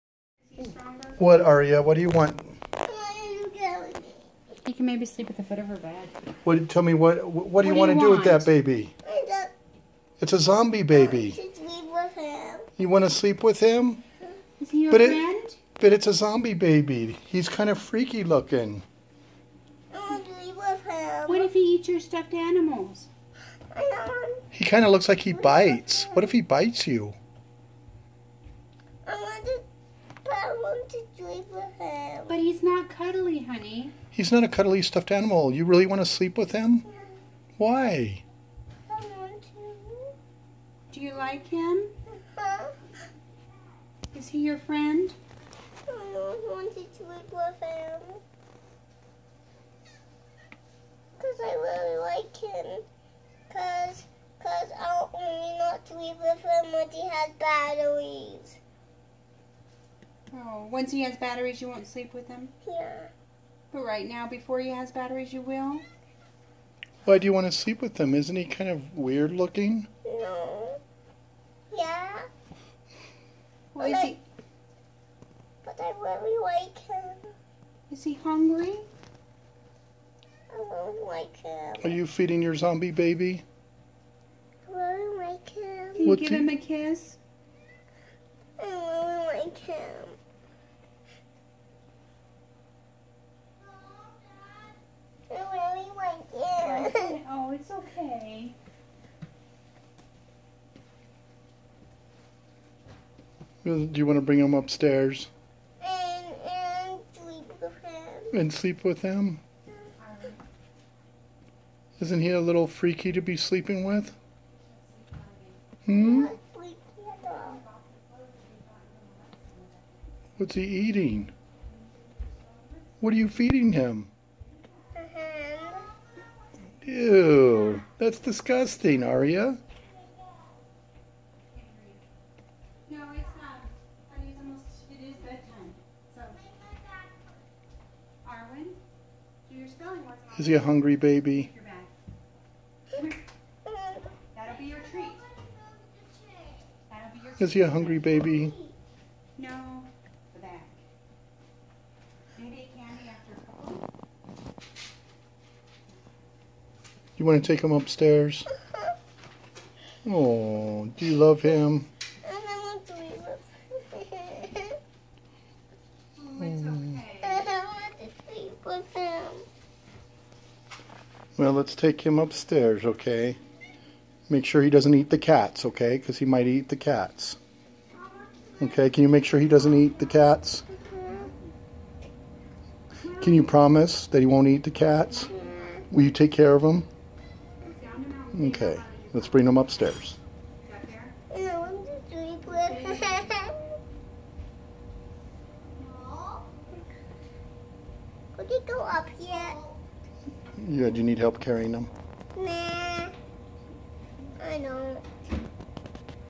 Toddler loving zombie baby
Toddler falls in love with a zombie baby deco